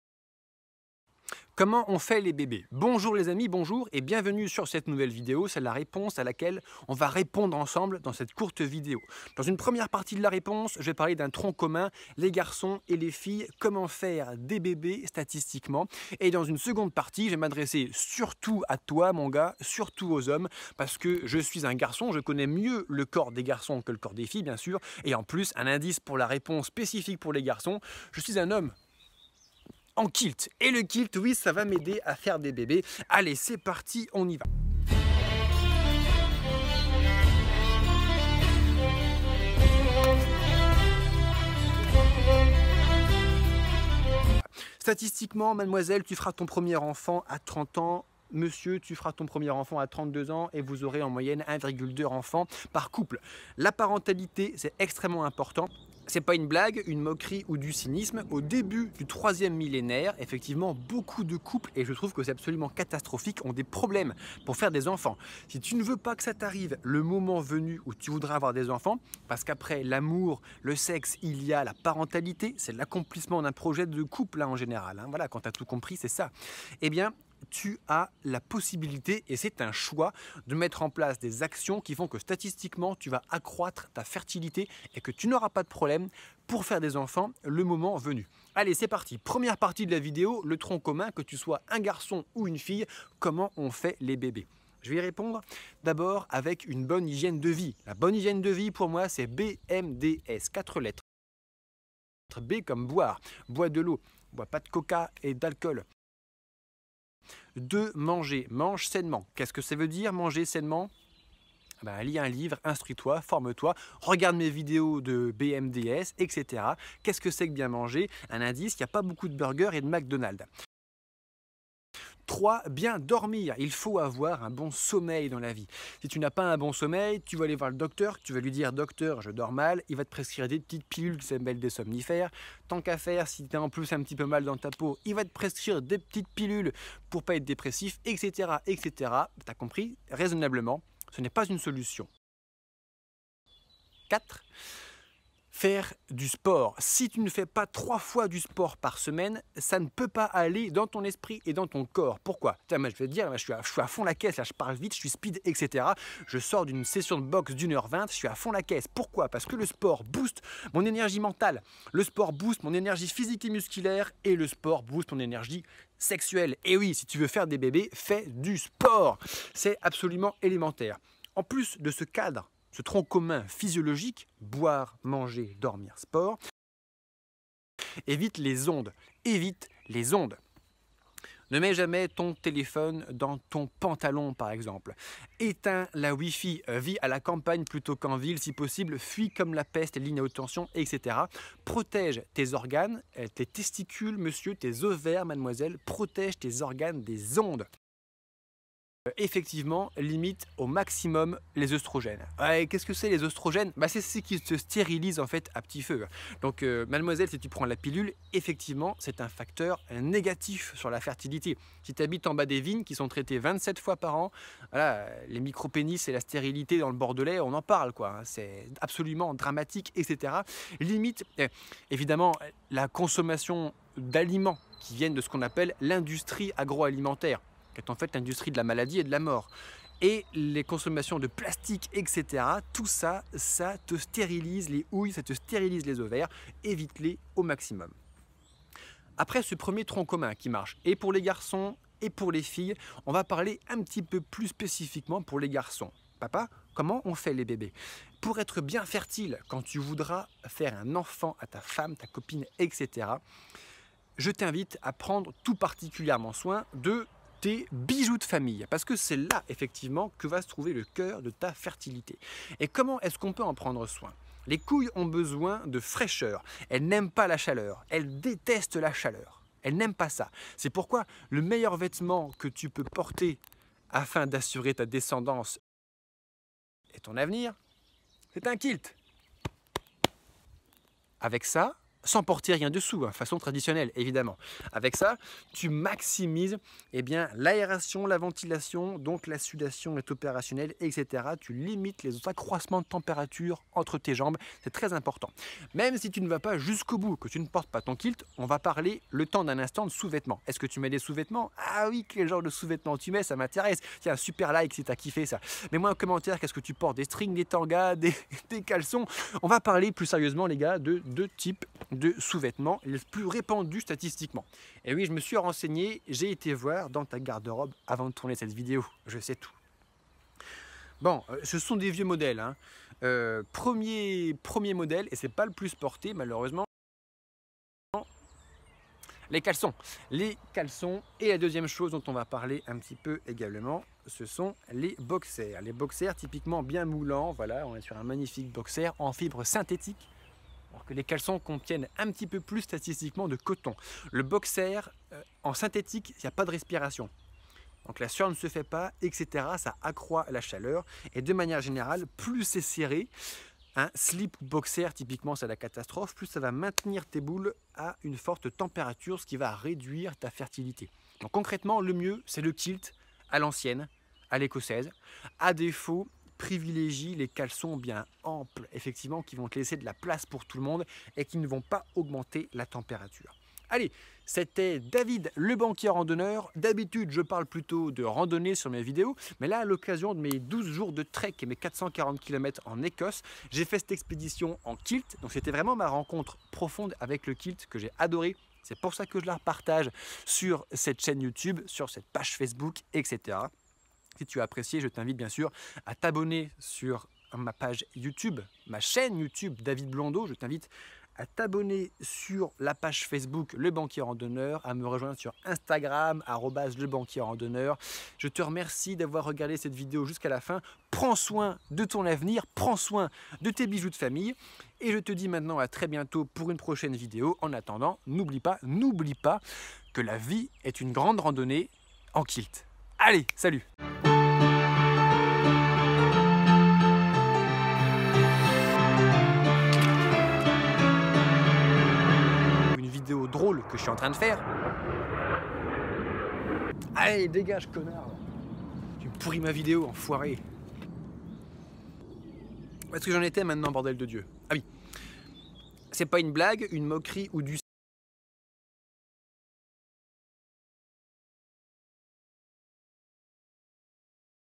Comment on fait les BEBES en 8 étapes
Comment on fait les Bébés ?
Trop de couples rencontre des difficultés à faire un enfant, alors l'ambassade d'Ecosse en France vous présente : le KILT !!!
Mentions Légales : L'humour ne fait de mal, fait rire, et met dans de bonnes dispositions pour faire l'amour (femme qui rit... ;) )

MON MATÉRIEL POUR TOURNER LES VIDÉOS :
- I phone 13 Mini 512 Go
- GoPro Hero8
- Drône DJI 2
- Trépied Manfrotto Compact Action